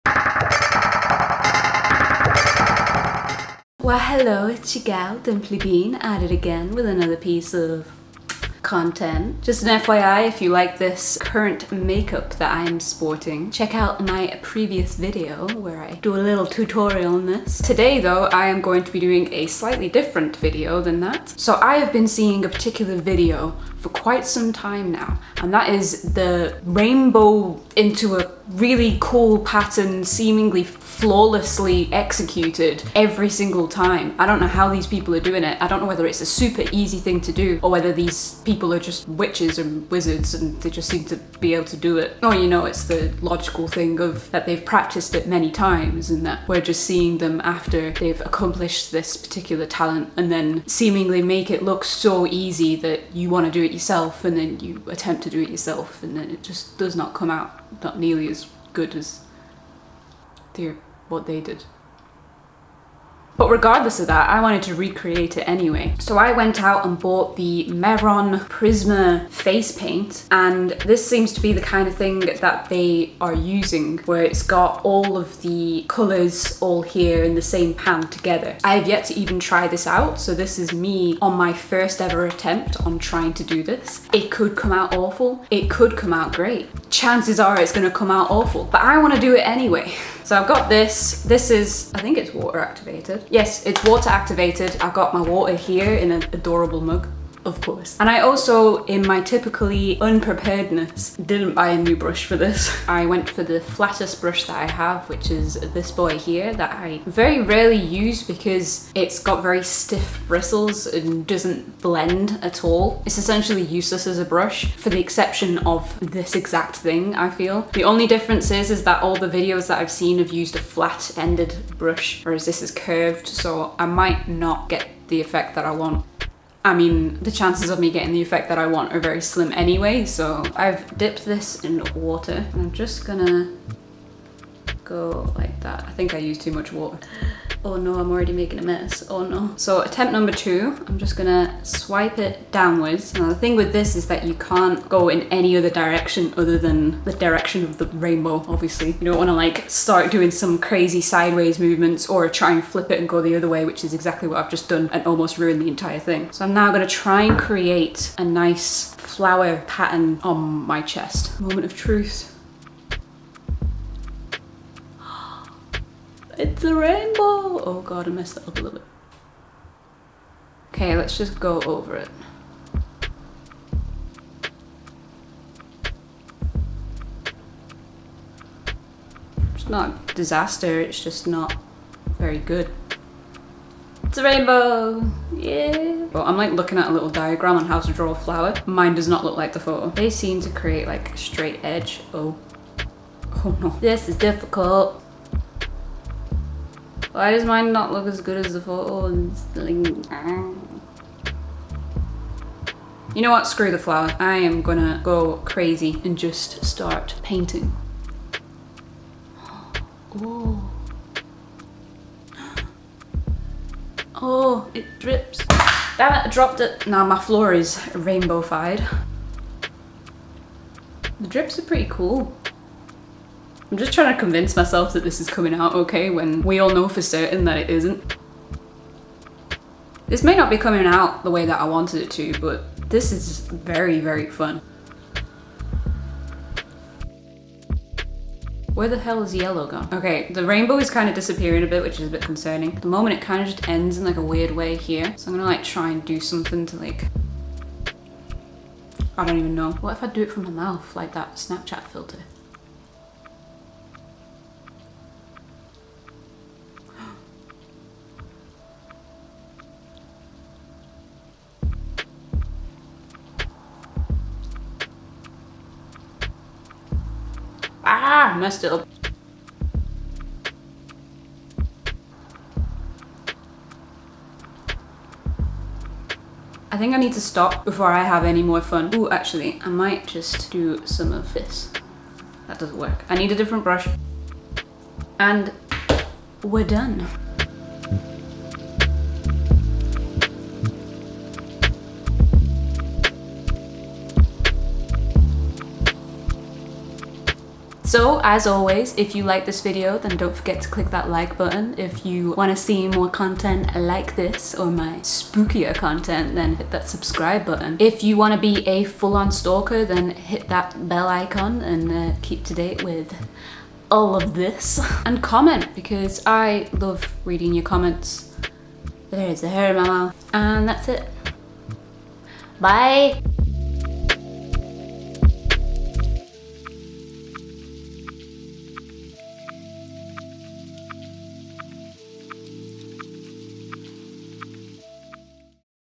I Turned Myself into a Rainbow 🌈
I bought a new thing, I like the new thing.

Let me know down in the comments what content you'd like to see from me!

✨ Products Used ✨
Mehron Paradise Prisma Cake

▷ Join the ZomBean Crew...

▷ Other Videos to Satisfy Those Dimple Cravings...